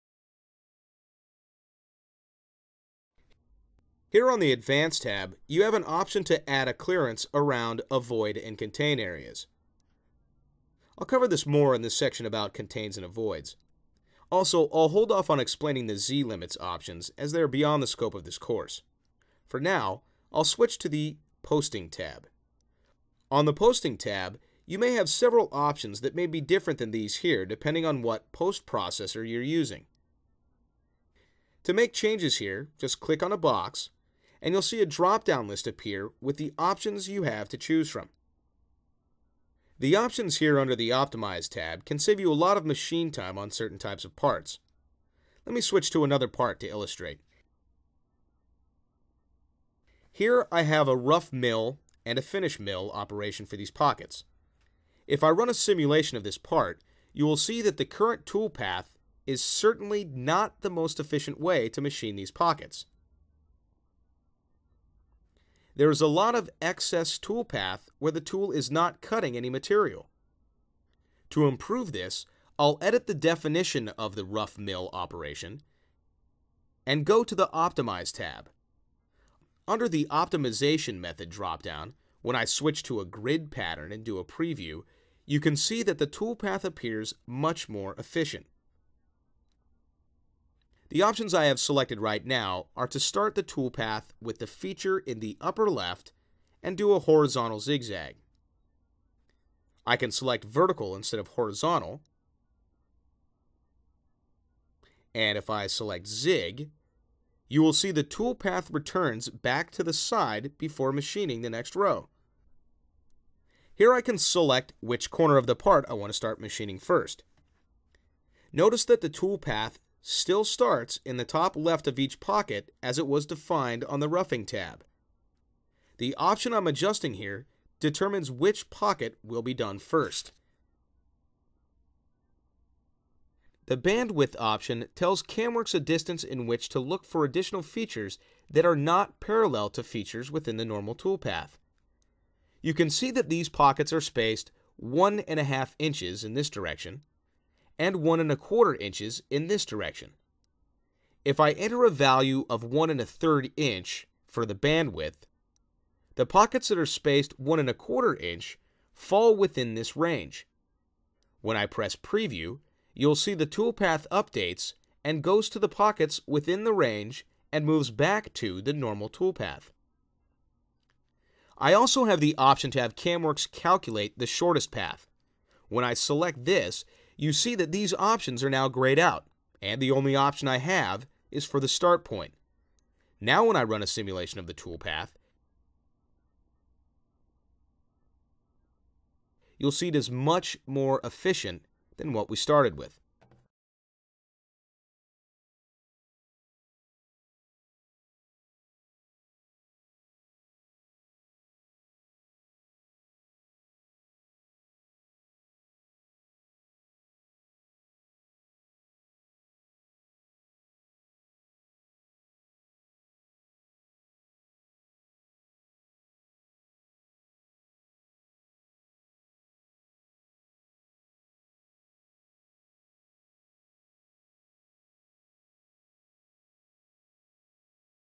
CAMWorks Tutorial by SolidProfessor Optimizing Toolpaths
This SolidProfessor video explains how to use options for optimizing toolpaths.

Sign up for a free membership to experience how SolidProfessor can help you design with confidence